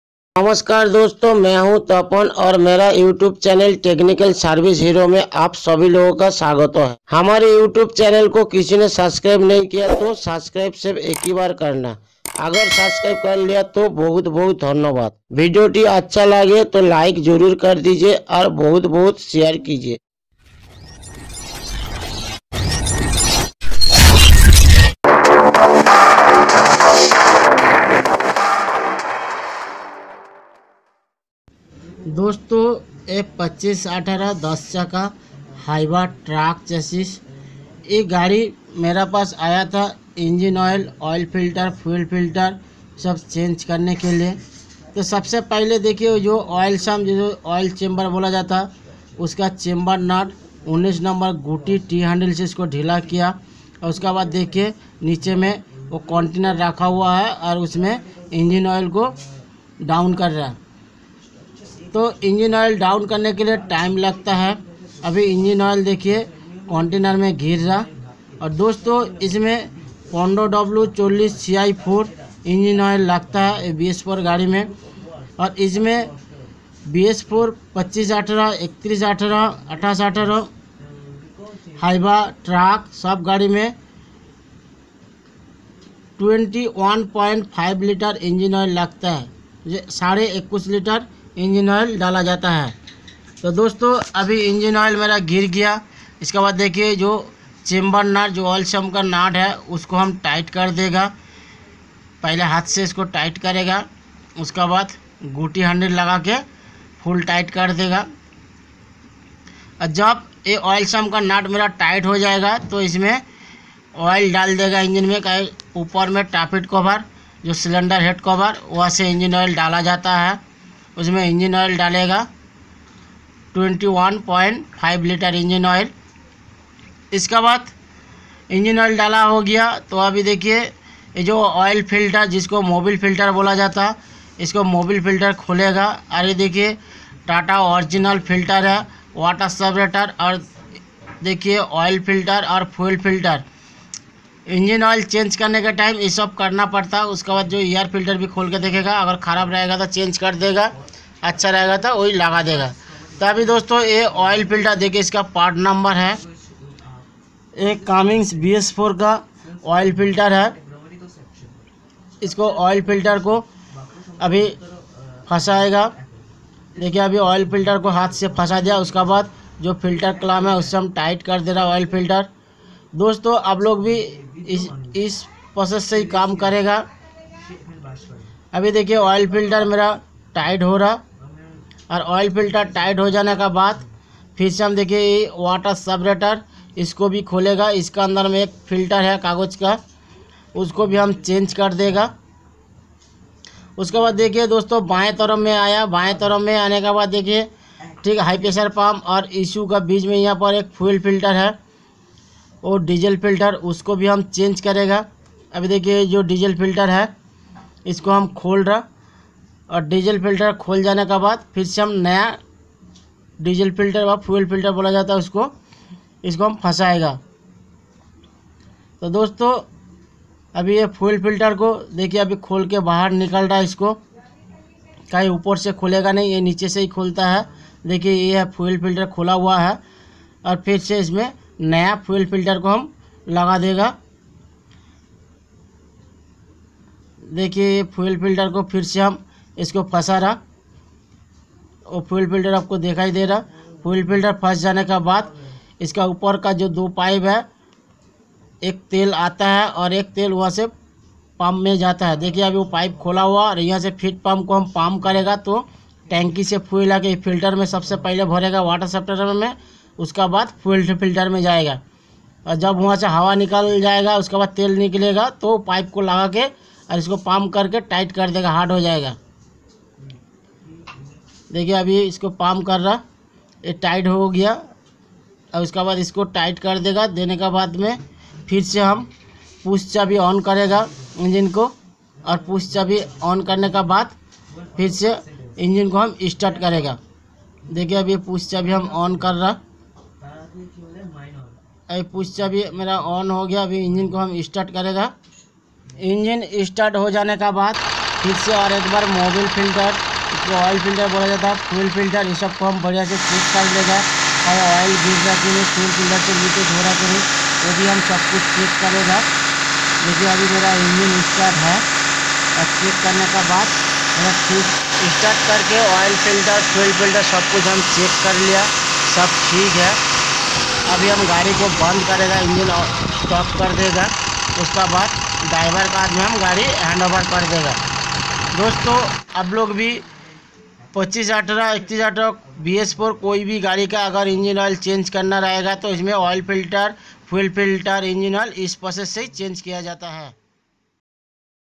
Tata 2518Cm Bs4-1618Cm Bs4 Hyva Tipper Track Engine Oil And Oil Filter Fuel Filter Air Filter Change
Technical service hero .

Technical service hero .

Hello Friends Aaj Ka Is Video Ma Hum Dikhana Charahe Hai .

Tata 2518Cm Bs4 Hyva Engine Oil And Oil Filter Fuel Filter Air Filter Change .